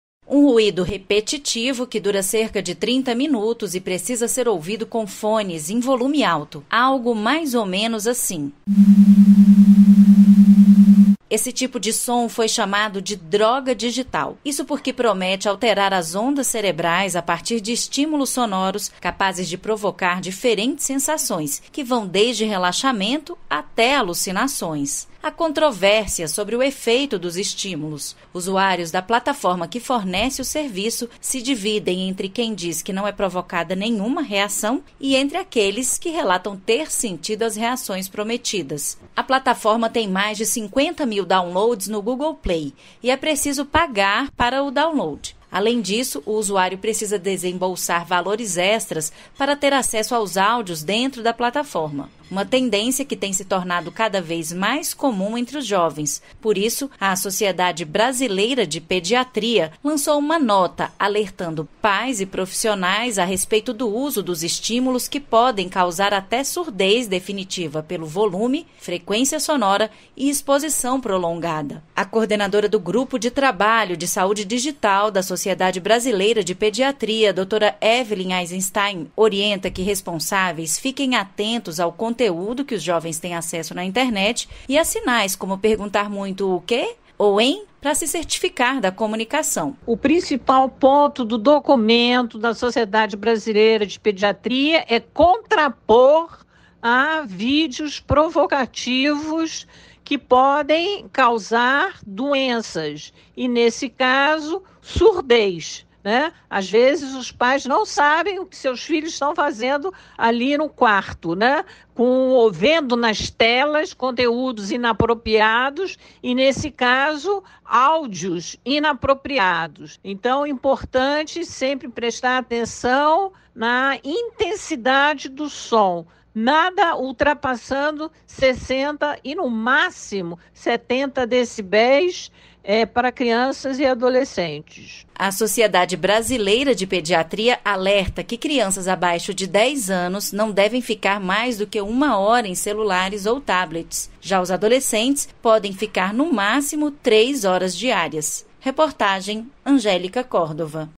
MÉDICOS ALERTAM PARA OS RISCOS DAS ‘DROGAS DIGITAIS’ PARA AS CRIANÇAS E ADOLESCENTES
| BRASIL |